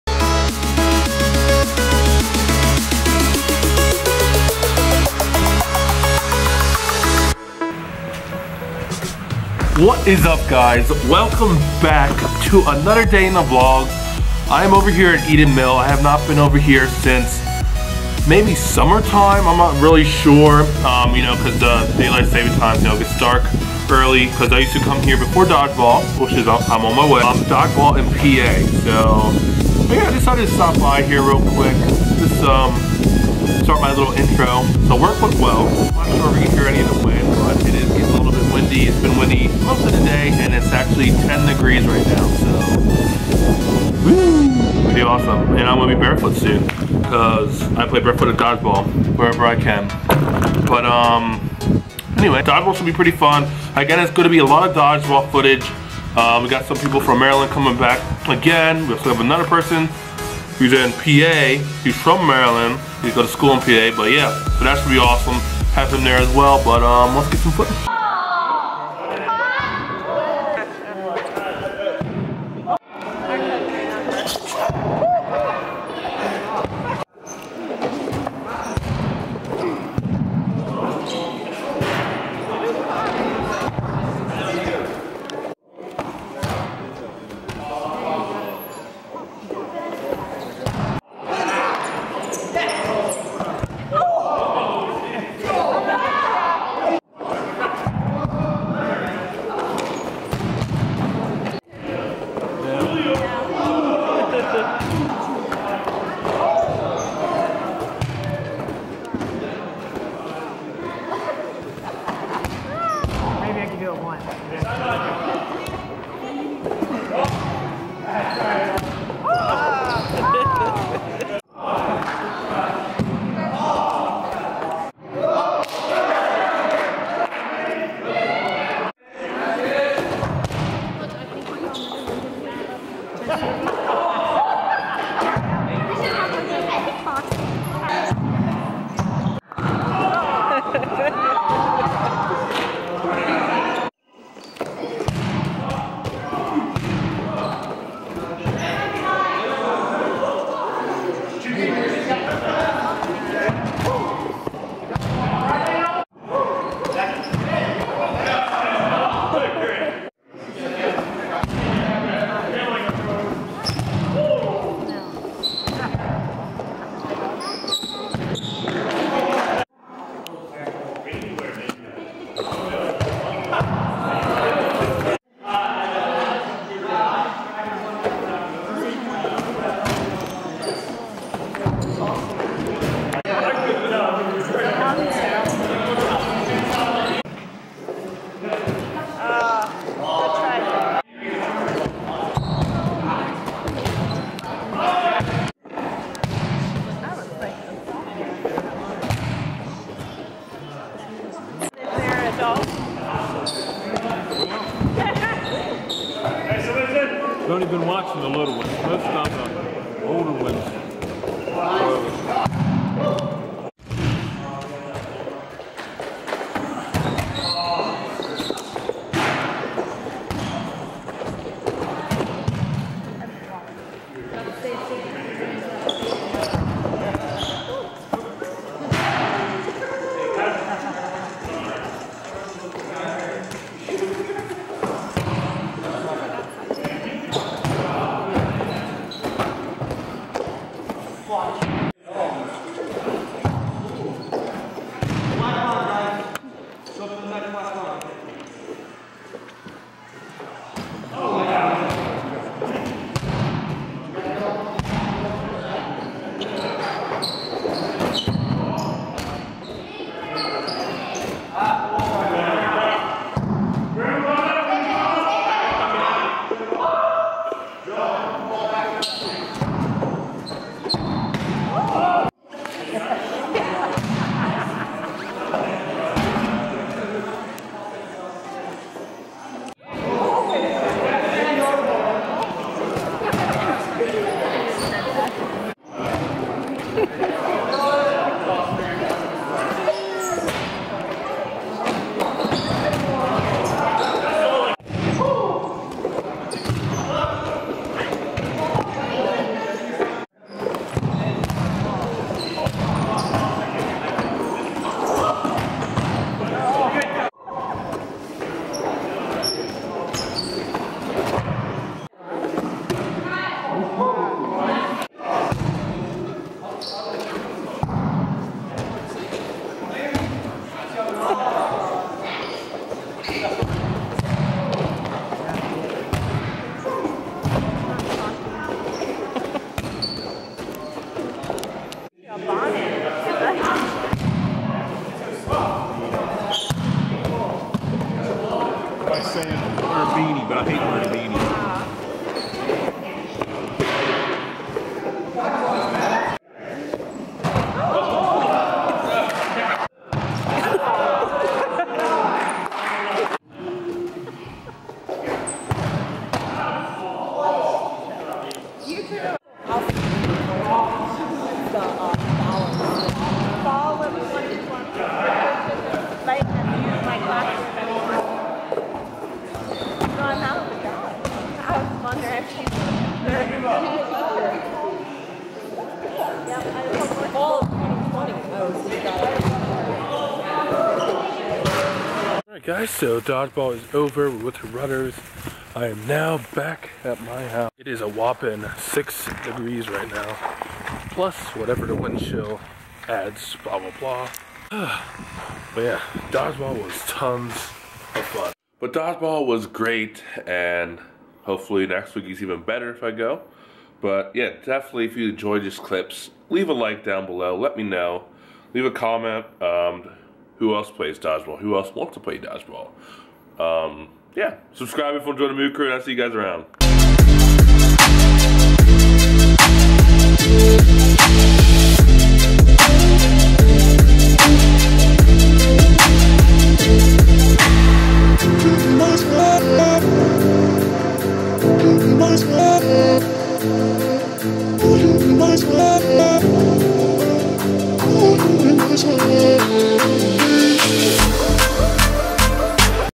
Ultimate Game of Dodgeball
Hello Everyone!! So im back with some more footage from dodgeball in Pennsylvania! We had some really great games and Sasimo got some awesome footage!

Follow Me**
moo45